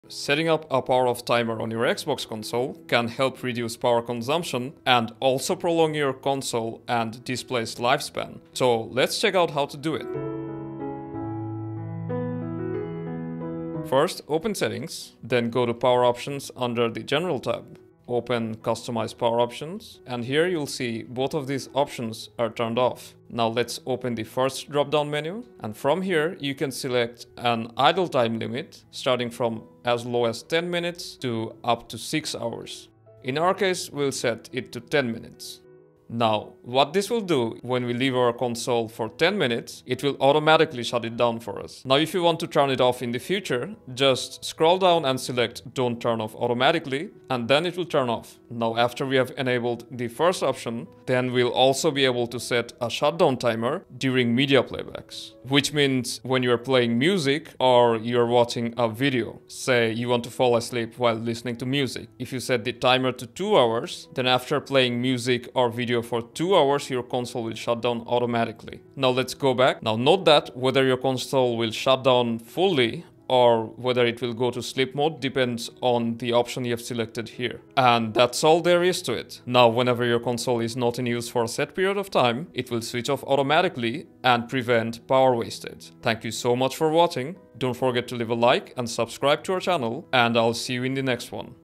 How to SET Xbox One Sleep Timer! AUTO Shutdown After Inactivity
Do you want your Xbox One Series S and X to Turn Off automatically after being inactive for some time, so it does not keep running for hours and keep consuming unnecessary energy? In this video, we will show you how to set up a sleep timer on Xbox One for it to auto-shutdown after the allotted time.

We will also show you how to stop the Xbox One from shutting down automatically while watching videos or movies or turn off the sleep timer automatically.

0:00 Xbox One Auto shutdown timer
0:14 Setup sleep timer for Xbox
0:41 Turn Off the Sleep timer
0:53 Enable Auto Shutdown Timer
1:09 Change Power Options